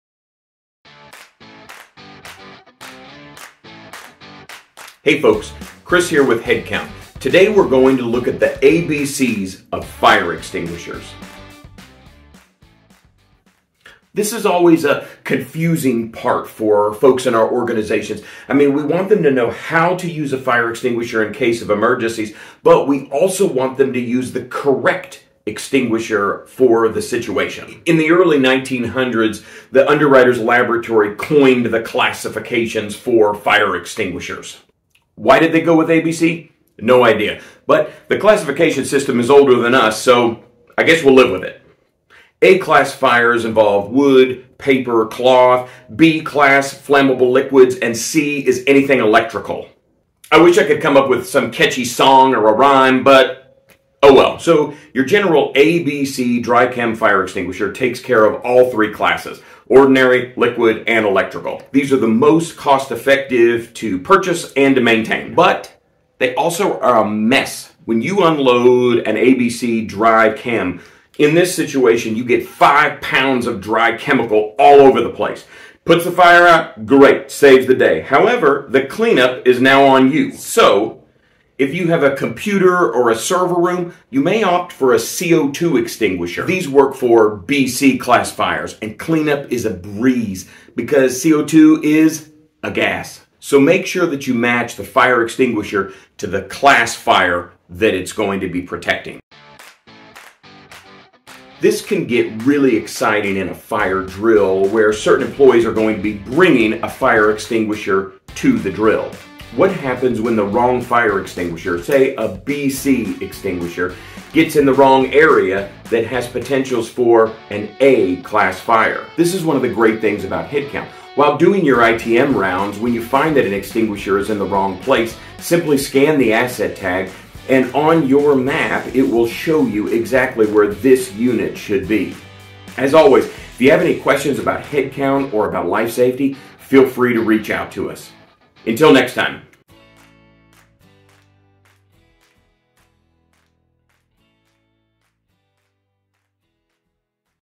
HeadScratcher - The ABC's of Fire Extinguishers
We look at the classification system and how to choose the proper fire extinguisher for your setting